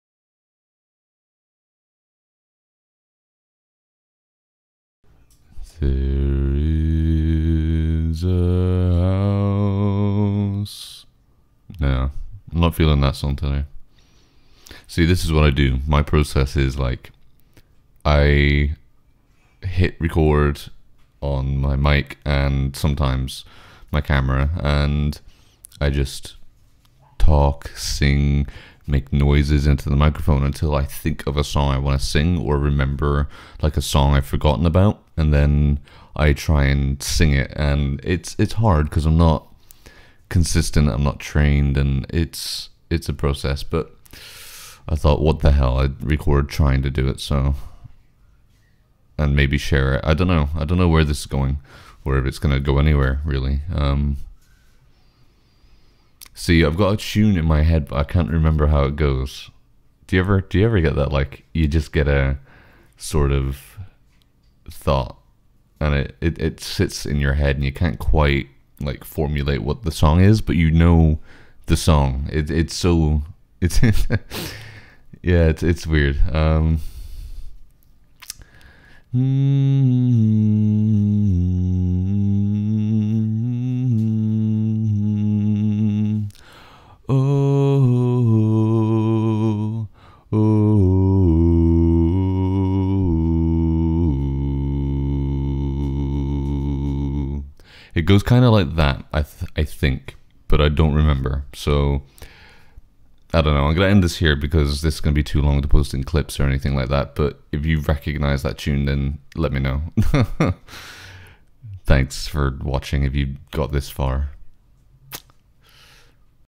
Behind The Scenes | How I Figure Out What Songs To Sing
So - I like to sit down and sing into a microphone until I find a tune, song or melody that really sticks with me. I don't always keep, use or even think about the recordings since they're just for me! - I decided to record this session and right at the end I started explaining a bit about why I do it. So I thought I'd cut it down and share it on the channel!